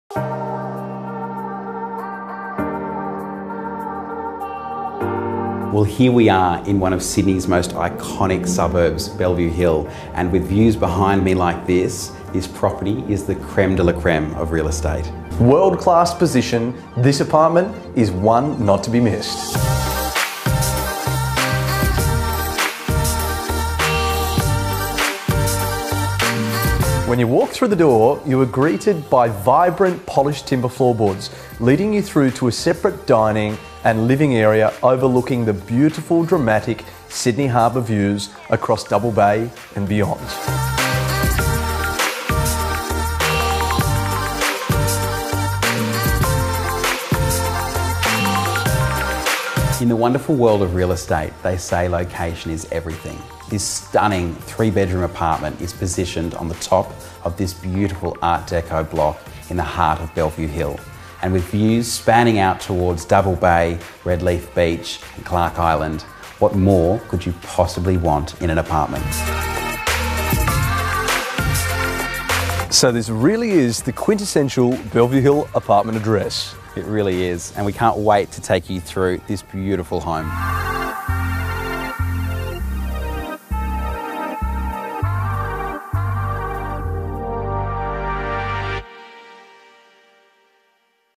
14 Fairfax Road, Bellevue Hill
. Interiors are bathed in natural light, offering a flexible layout that can be adapted to suit individual requirements. Sharing no common walls, it's set in a boutique Art Deco block with formal reception areas and charming garden, while additional highlights include plentiful accommodation and potential for parking. Double Bay Village and city buses are a short stroll away.